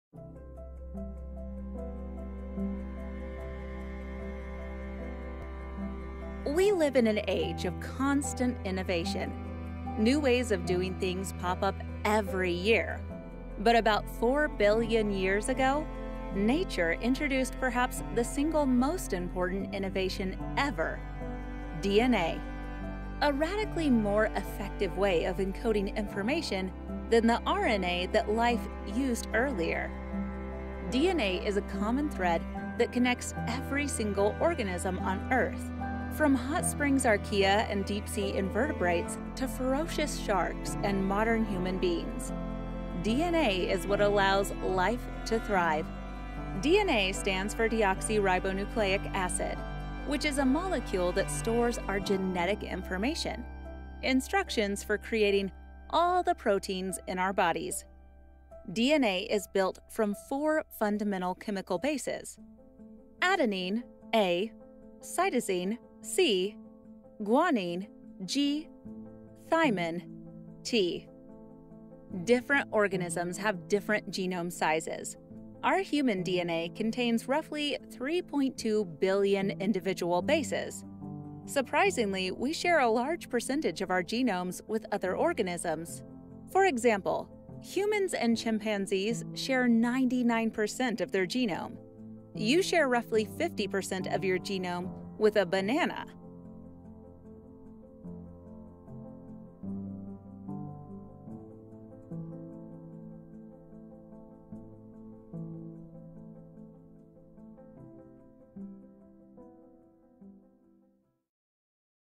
DNA & the Human Genome (Introduction)
We live in an age of constant innovation. New ways of doing things pop up every year. But around 4 billion years ago, nature introduced perhaps the single most important innovation ever: DNA, a radically more effective way of encoding information than the RNA that life used earlier.

DNA is a common thread that connects every single organism on Earth, from hot spring archaea and deep-sea invertebrates to ferocious sharks and modern human beings. DNA is what allows life to thrive.

What Is DNA?

DNA stands for deoxyribonucleic acid. It is a molecule that stores our genetic information—instructions for creating all the proteins in our bodies.
DNA is built from four fundamental chemical bases:

Adenine (A)
Guanine (G)
Thymine (T)

Different organisms have different genome sizes—our human DNA contains roughly 3.2 billion individual bases. Surprisingly, we share a large percent of our genome with other organisms. For example, humans and chimpanzees share 99 percent of their genome. You share roughly 50 percent of your genome with a banana.

Are you a health science educator?
Try our online health science course content for free!